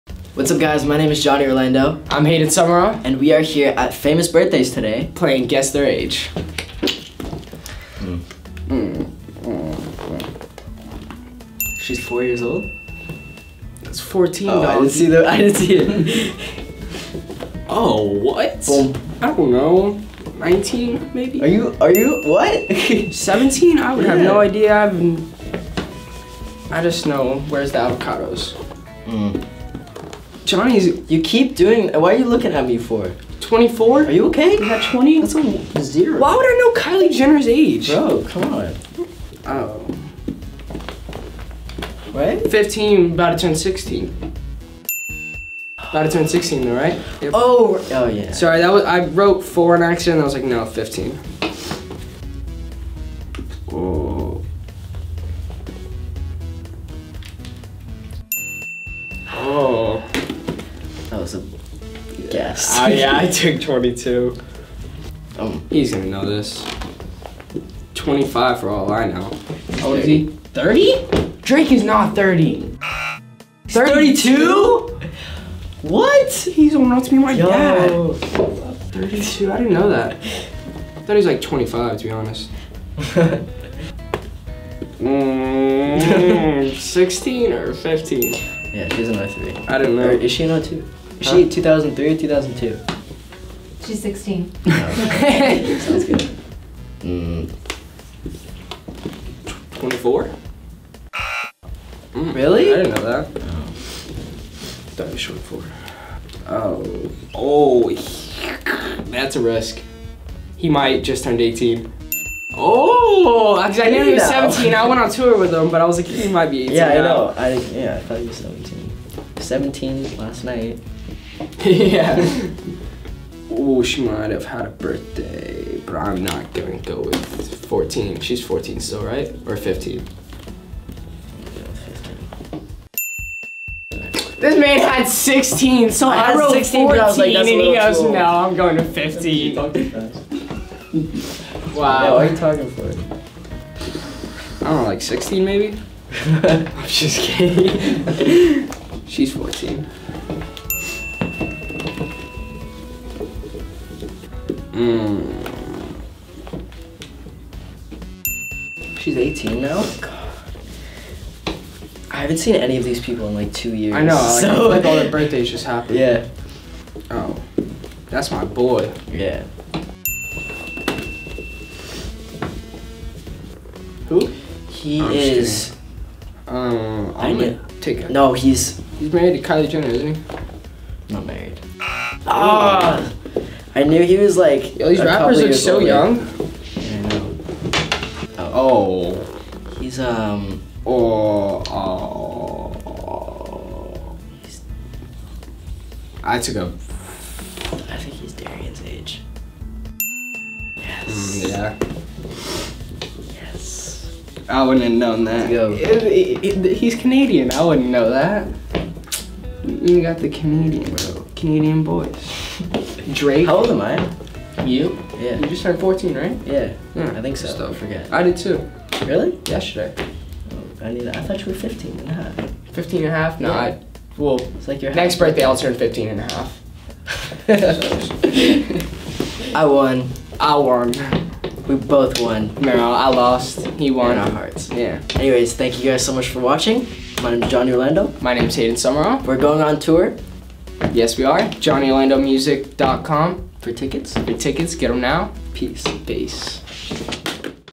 Johnny Orlando vs. Hayden Summerall - Guess Their Age
Johnny and Hayden stopped by the Famous Birthdays office on April 5th, 2019 to play a game of Guess Their Age